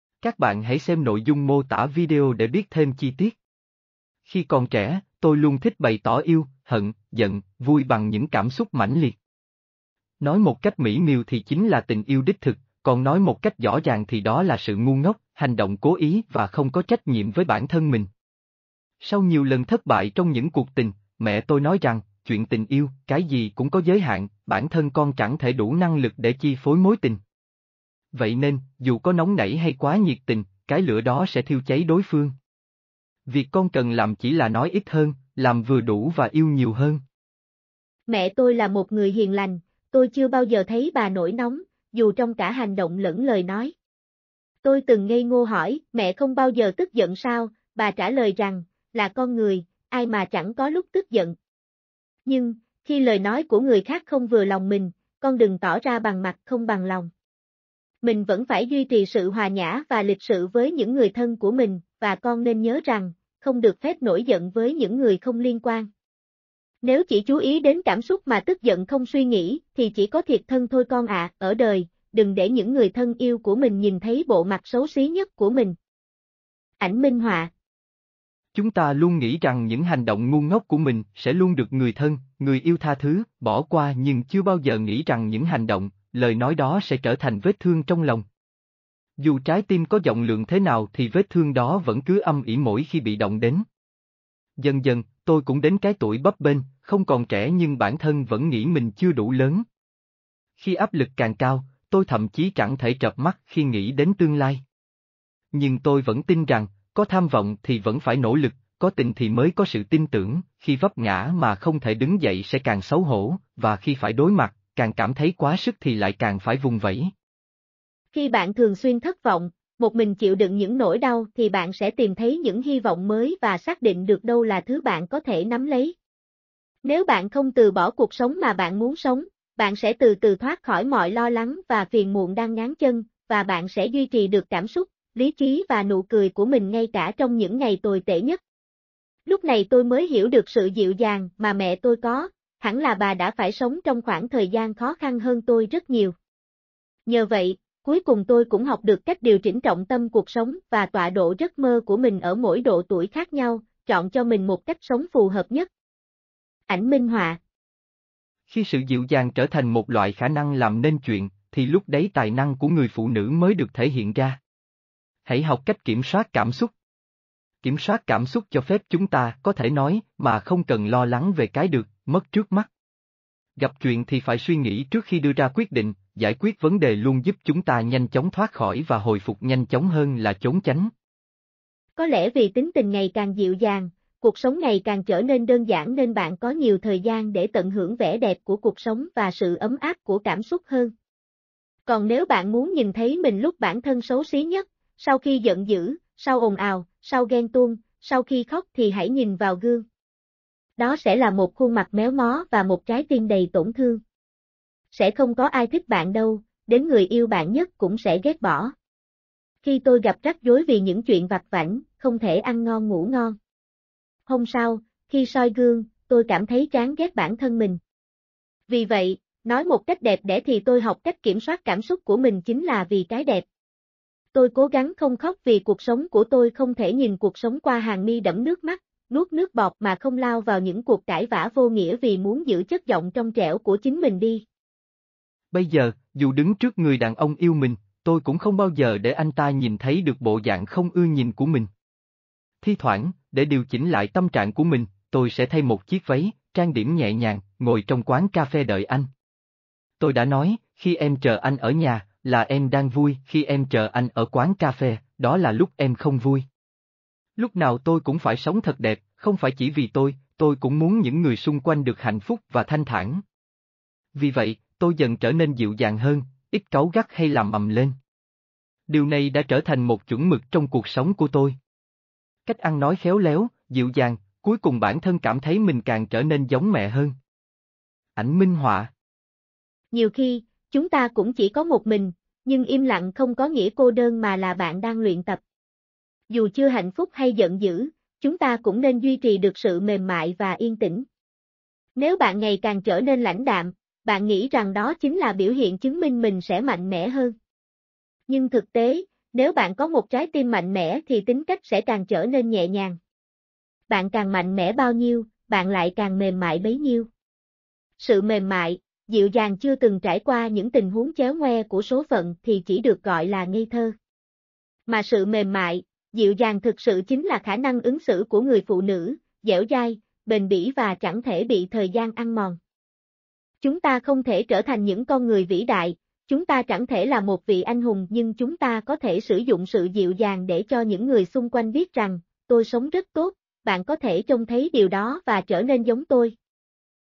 Tại sao những cô gái xung quanh đều dịu dàng, còn tôi thì không?
Khi còn trẻ, tôi luôn thích bày tỏ yêu, hận, giận, vui bằng những cảm xúc mãnh liệt. Nói một cách mỹ miều thì chính là tình yêu đích thực, còn nói một cách rõ ràng thì đó là sự ngu ngốc, hành động cố ý và không có trách nhiệm với bản thân mình. Sau nhiều lần thất bại trong những cuộc tình, mẹ tôi nói rằng: “Chuyện tình yêu, cái gì cũng có giới hạn, bản thân con chẳng thể đủ năng lực để chi phối mối tình. Vậy nên, dù có nóng nảy hay quá nhiệt tình, cái lửa đó sẽ thiêu cháy đối phương. Việc con cần làm chỉ là nói ít hơn, làm vừa đủ và yêu nhiều hơn”.
Mẹ tôi là một người hiền lành, tôi chưa bao giờ thấy bà nổi nóng, dù trong cả hành động lẫn lời nói. Tôi từng ngây ngô hỏi “Mẹ không bao giờ